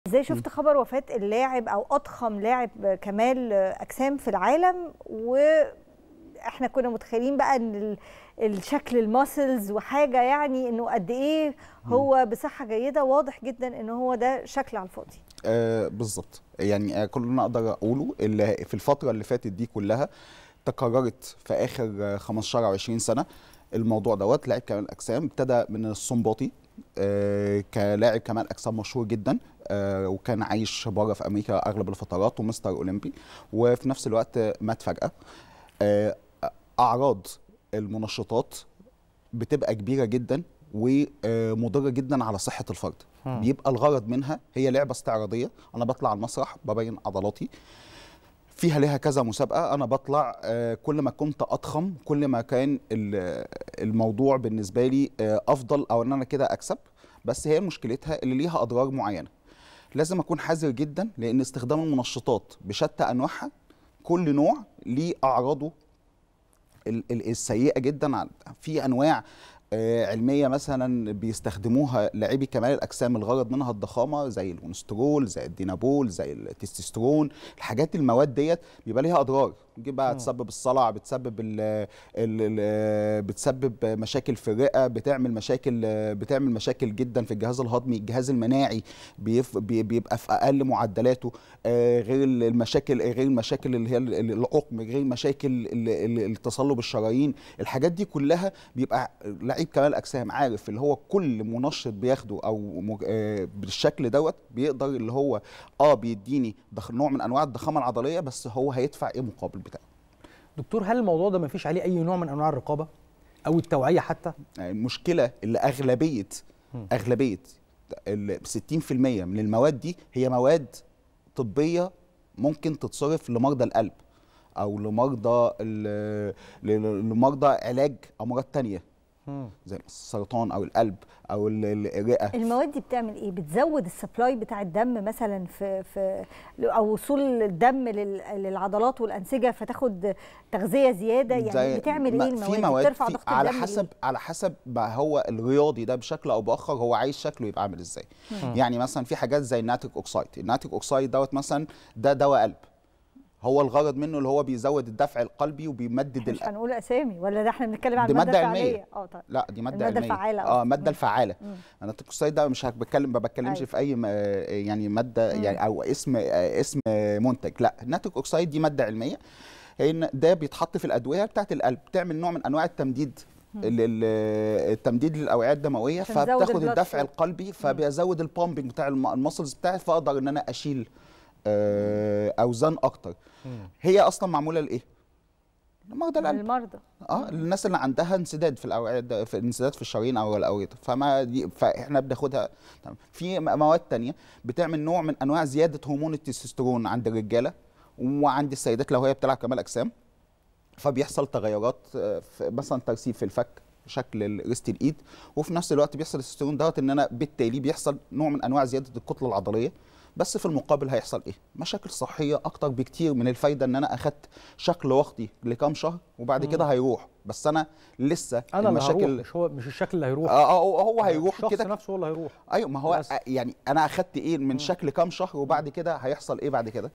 تفاصيل خاصة عن أسباب وفاة أضخم لاعب كمال أجسام في العالم.. هنعرفها مع د. حمدي الأمين أستاذ التغذية
صباح الخير يا مصر | تفاصيل خاصة عن أسباب وفاة أضخم لاعب كمال أجسام في العالم.. هنعرفها مع د. حمدي الأمين أستاذ التغذية الرياضية